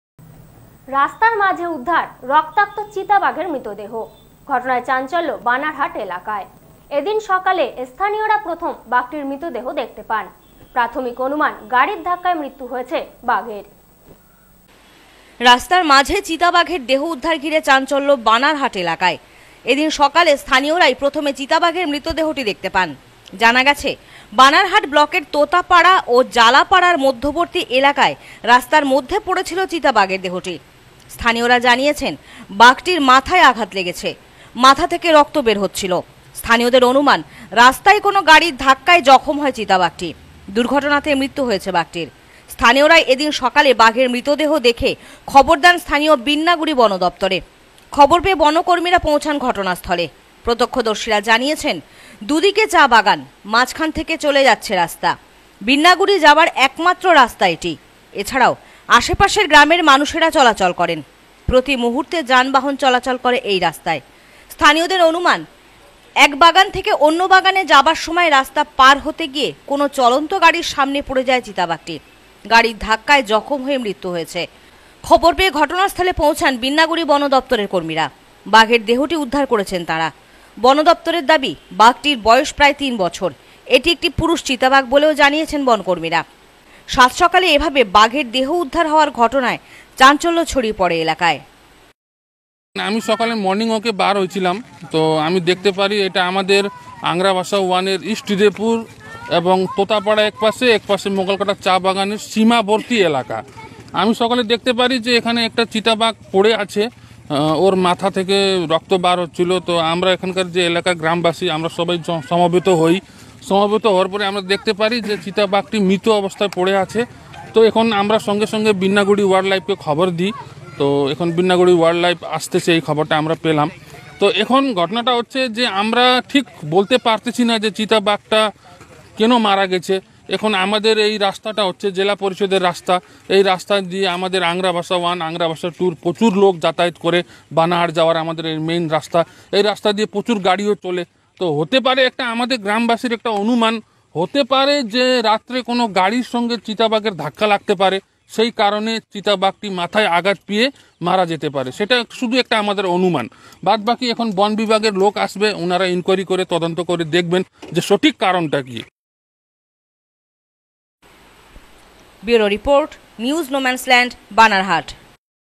চিতাবাঘের মৃতদেহ। ঘটনায় চাঞ্চল্য
রাস্তার মাঝে উদ্ধার রক্তাক্ত চিতাবাঘের মৃতদেহ। ঘটনায় চাঞ্চল্য বানারহাট এলাকায়। এদিন সকালে স্থানীয়রাই প্রথম বাঘের মৃতদেহটি দেখতে পান। প্রাথমিক অনুমান গাড়ির ধাক্কায় মৃত্যু হয়েছে চিতাবাঘটির।